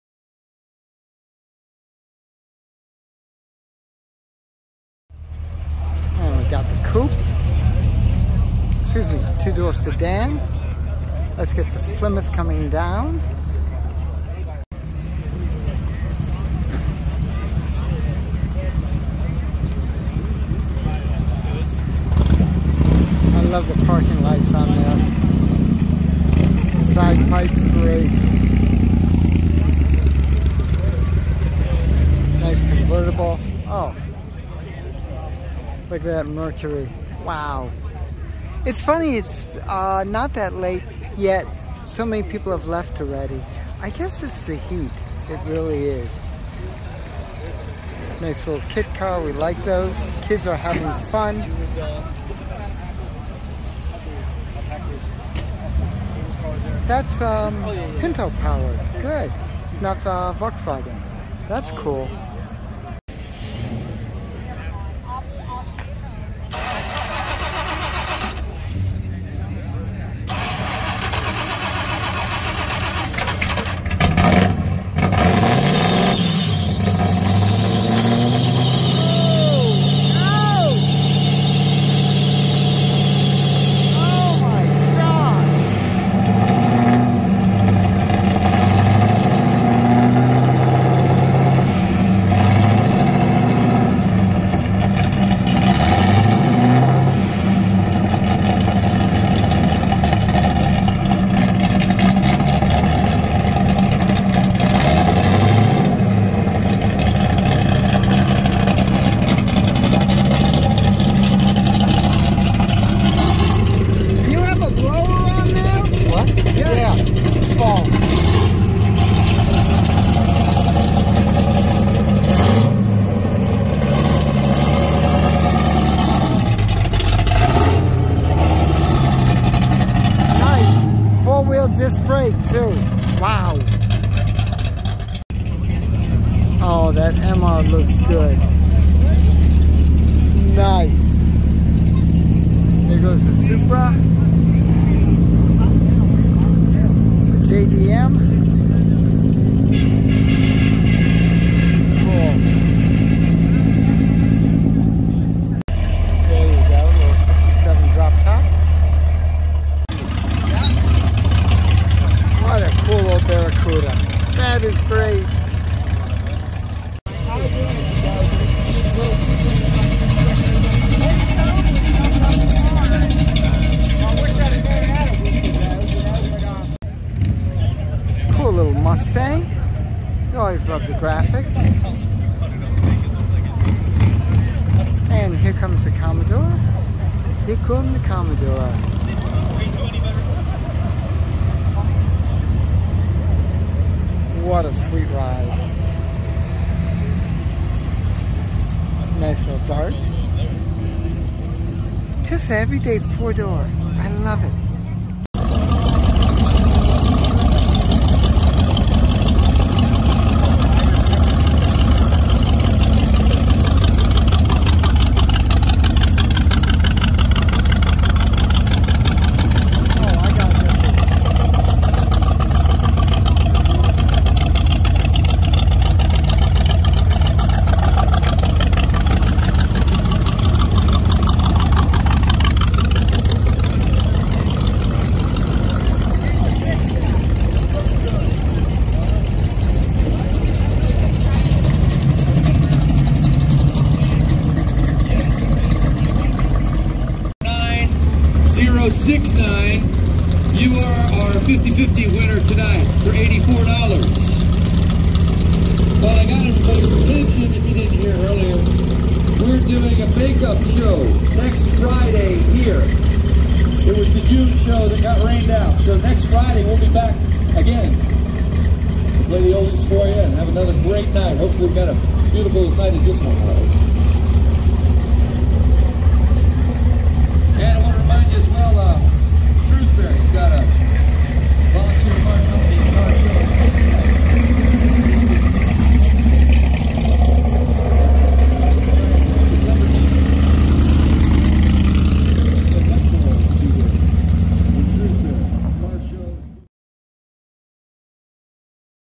Motor Menders Friday Night Cruise Aug 2022 Video 6
This classic car cruise is so well attended by everything from '32 Ford hot rods to '60's muscle cars and everything in between! For more photos, videos and your regional calendar of events,